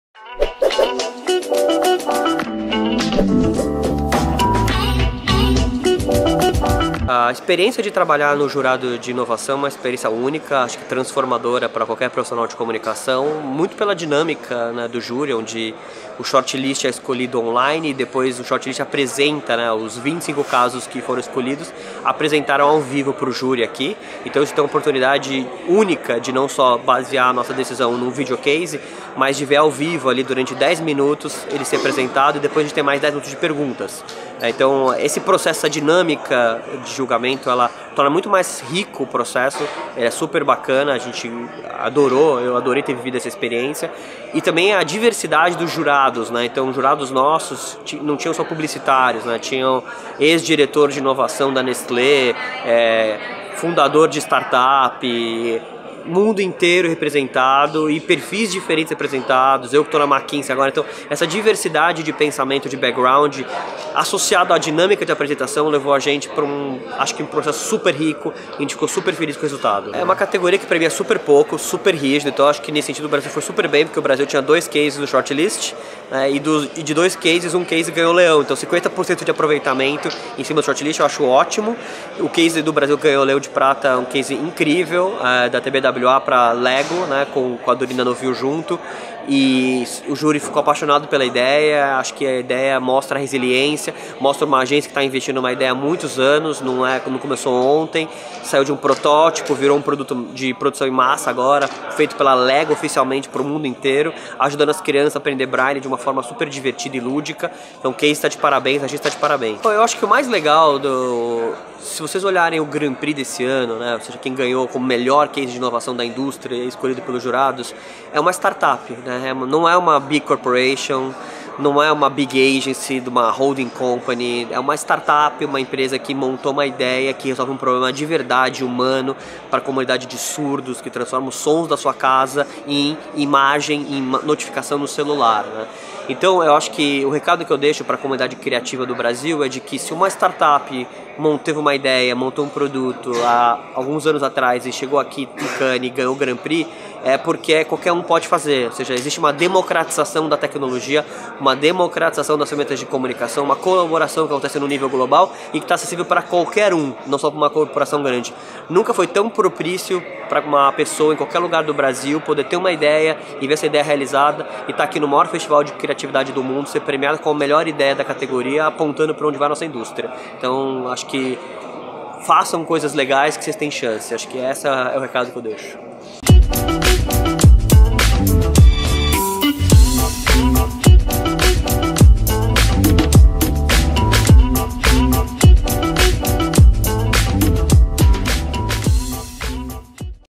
Entrevista com MARCELO TRIPOLI, VP Digital na Mckinsey (Cannes Lions 2019)
Marcelo Tripoli foi jurado em Cannes Lions 2019 na categoria Innovation